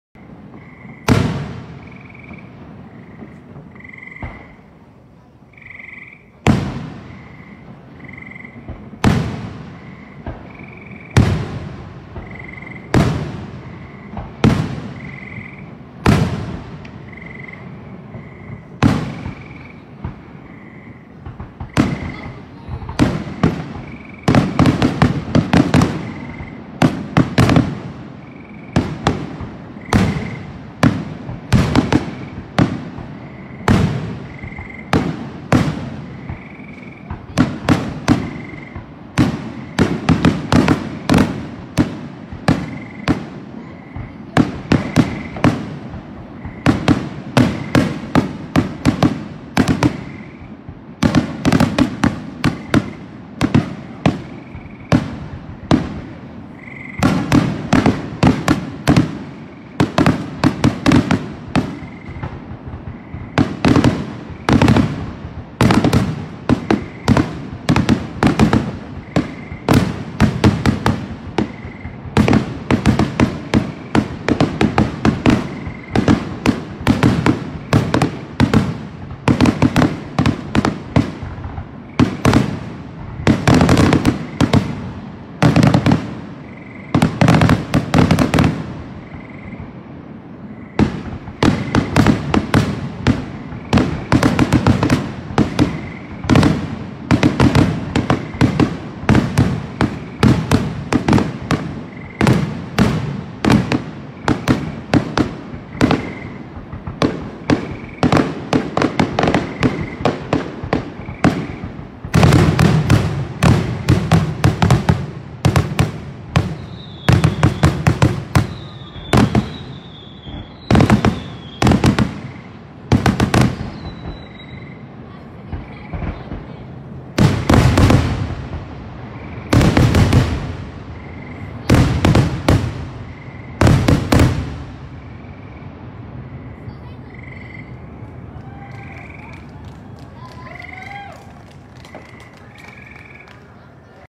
Grand finale from last nights firework display for Island Park in Schuylkill Haven, PA
Grand finale from last nights firework display we did for Island Park in Schuylkill Haven, PA 7/11/20.

Video is shared from the Schuylkill Haven Island Park facebook page.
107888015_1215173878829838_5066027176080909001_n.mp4